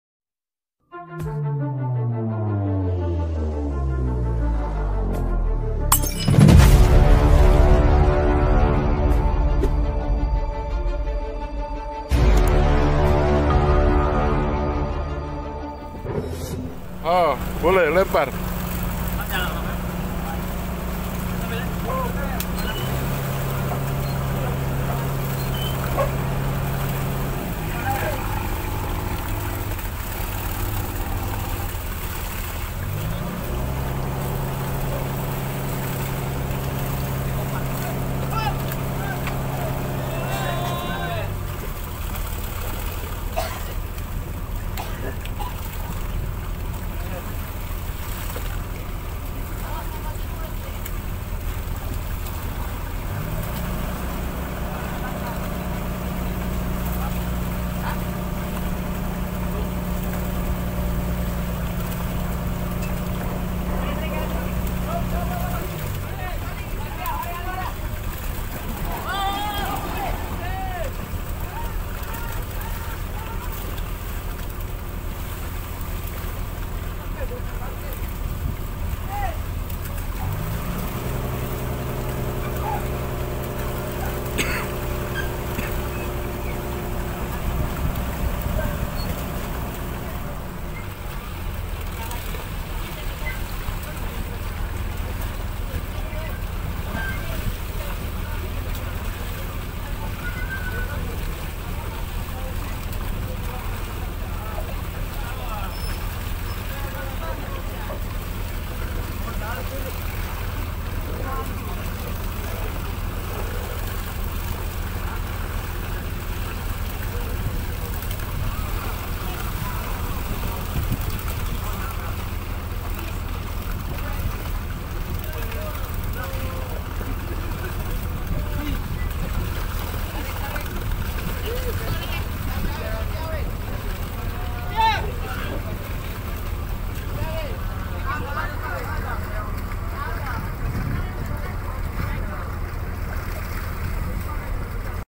MEMBERI MAKAN IKAN LUMBA-LUMBA DI LAUT
Video memberi makan ikan lumba-lumba di laut,
Ikan lumba-lumba ini selaluh hadir di setiap kapal jaring kerja caduk ikan di jaring kantong kapal power block , ikan lumba-lumba ini sangat pintar dan tau kalo kapal jaring lagi kerja pasti banyak makanan karna banyak ikan yang masuk alat power block pasti ikannya rusak dan di buang